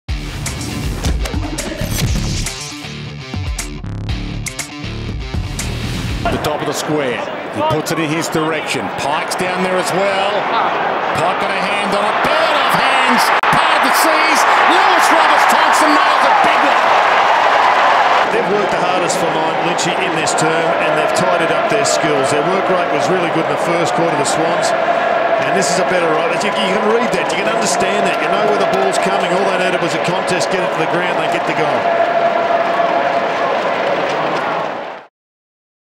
SwansTV: LRT in the square: Plays of the Day R5
LRT gets on the end of a Bird hand-pass and slots a goal from right in front